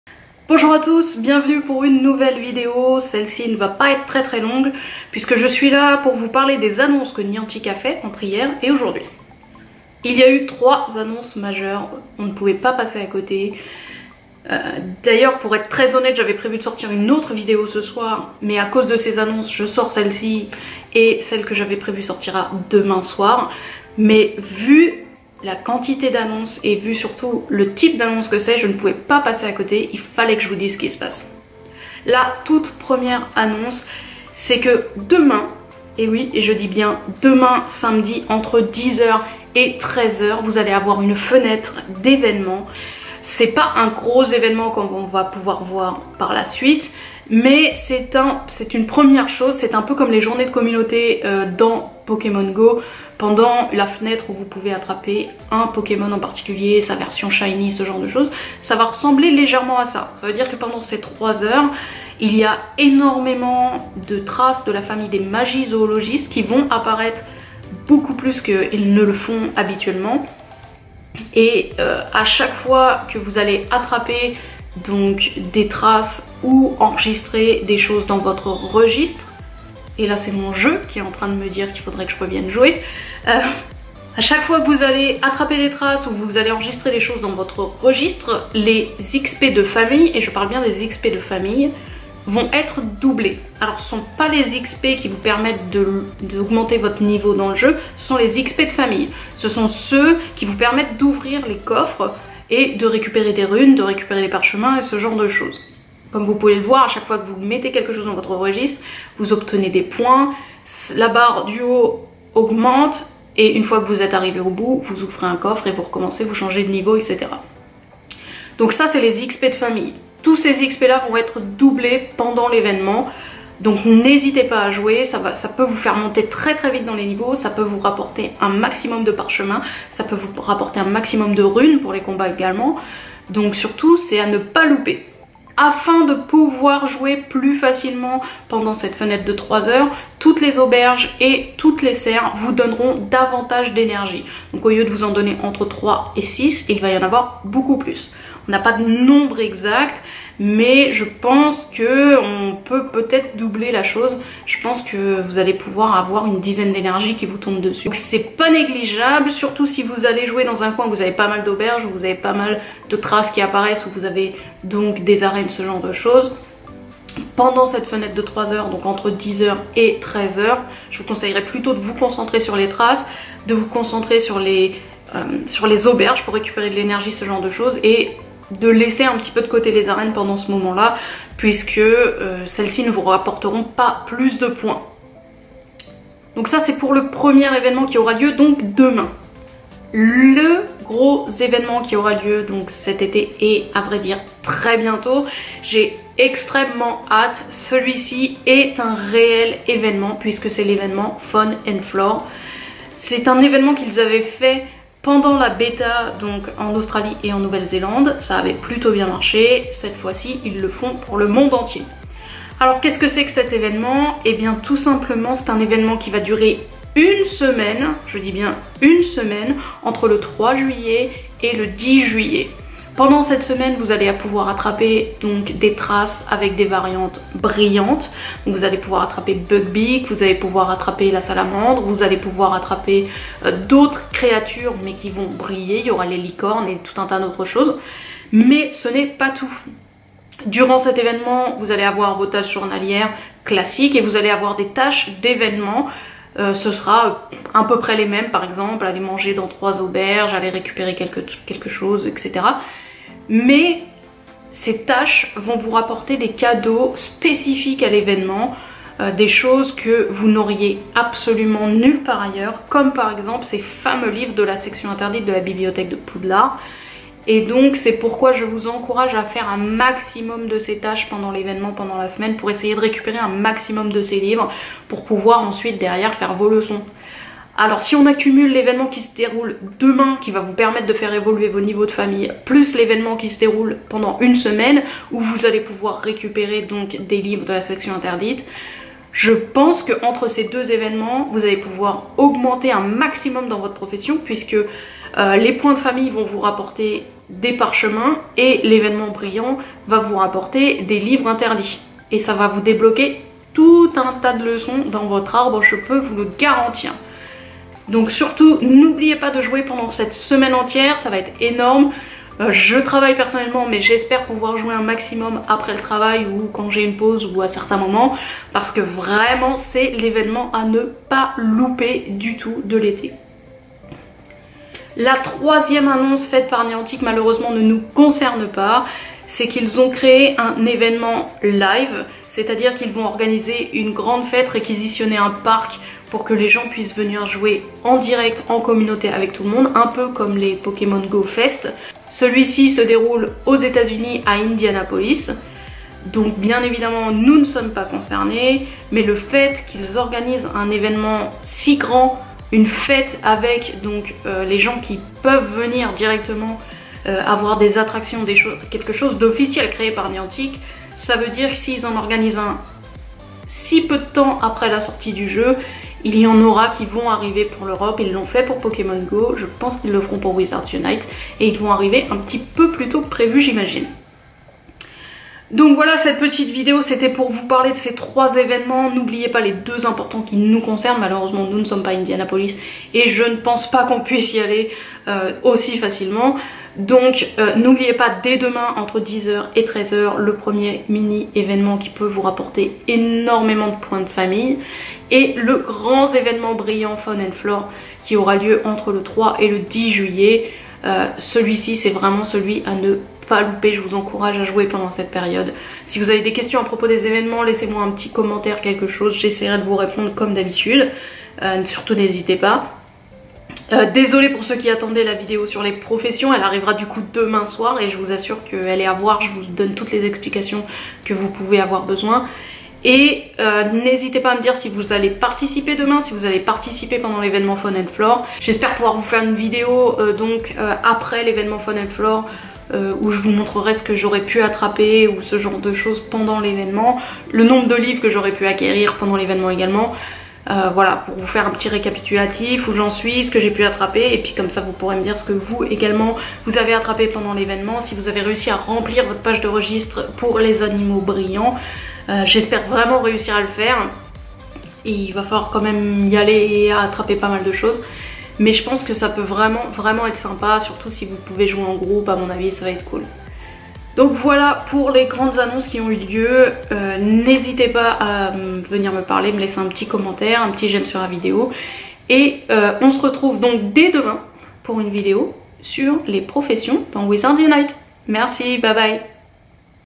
WIZARDS UNITE
Niantic a fait des annonces importante je ne pouvais donc pas ne pas vous en parler.

Etes vous prêt pour les événements spéciaux Wizards Unite? Si oui il vous suffit de regarder la vidéo pour tout savoir